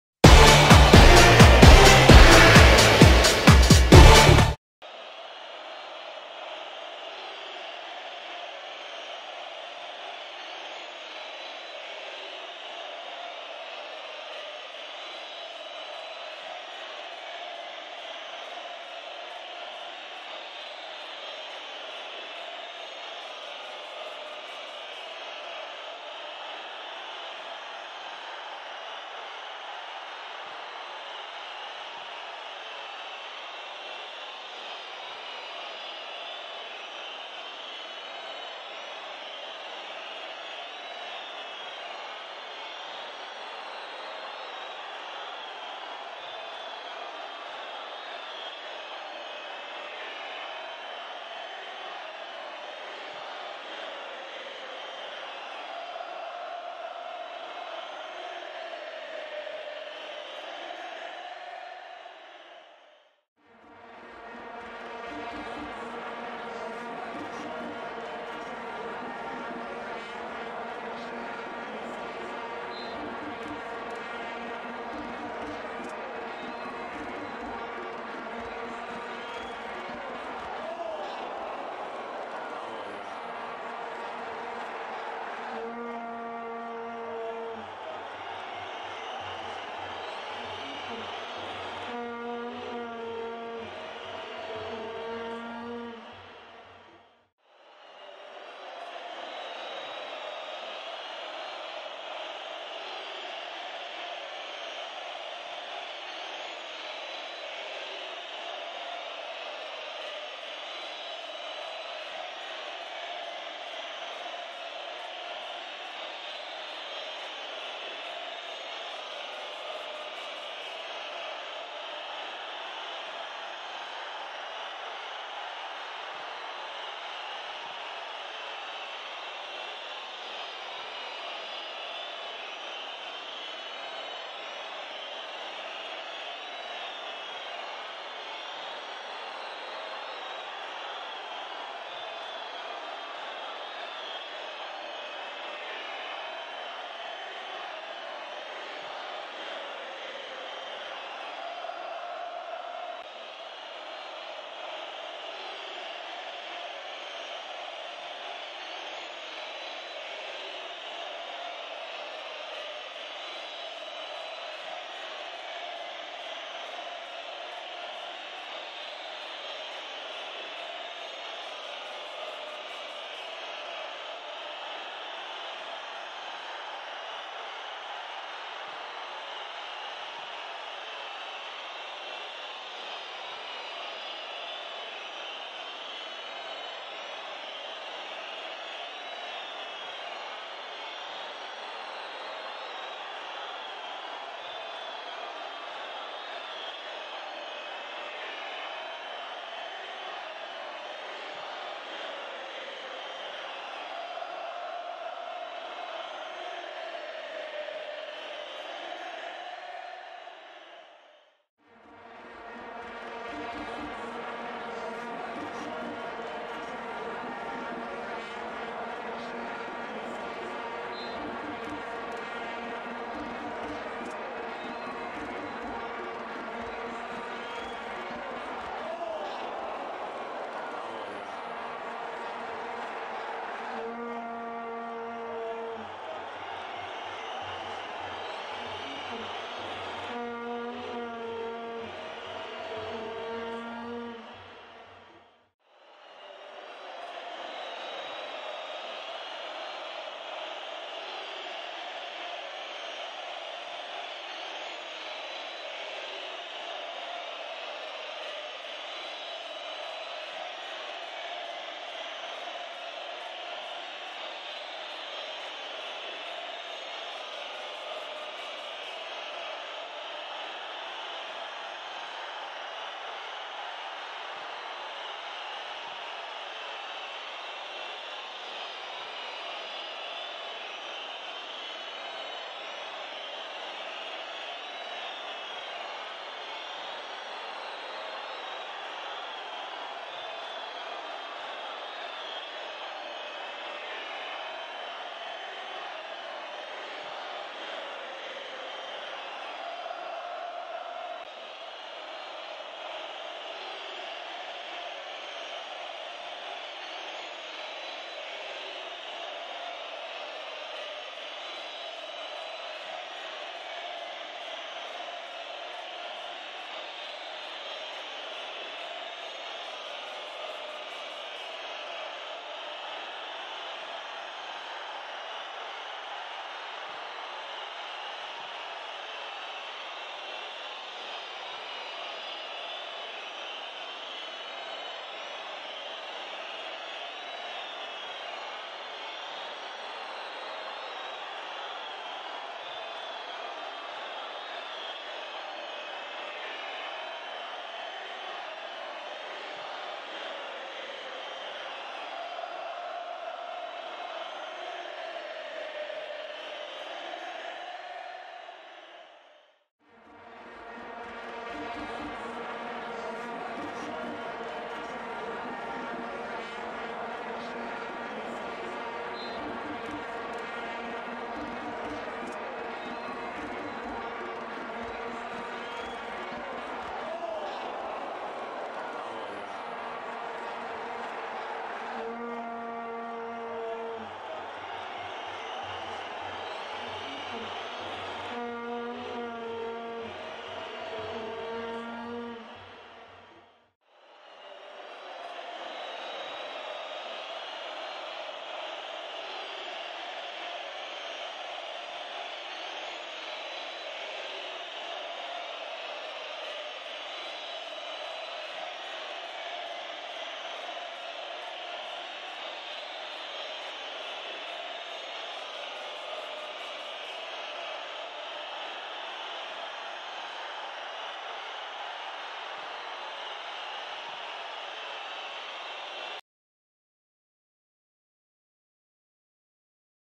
Achuapa 2-3 Sanarate Guatefutbol
Guatefutbol TV presenta el compacto del juego con más goles de la fecha 10
Por goles y polémicas, el juego entre Achuapa y Sanarate puede considerarse el partido más emocionante de la jornada 10 del Torneo Apertura de la Liga Nacional de Fútbol.